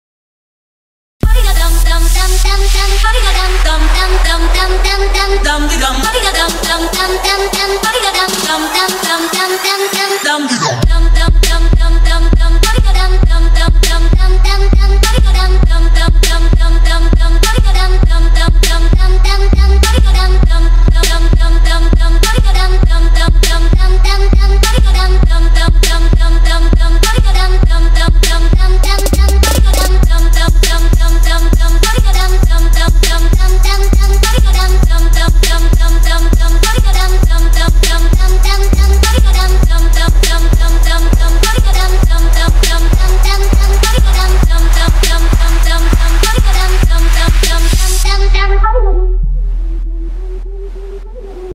Whats your favorite goal? #1 (by Viking TV)
1.Alexis Sanchez
2.Ronaldo
3.Pogba
4.Cisse
5.Sneijder
6.Adriano
7.Hulk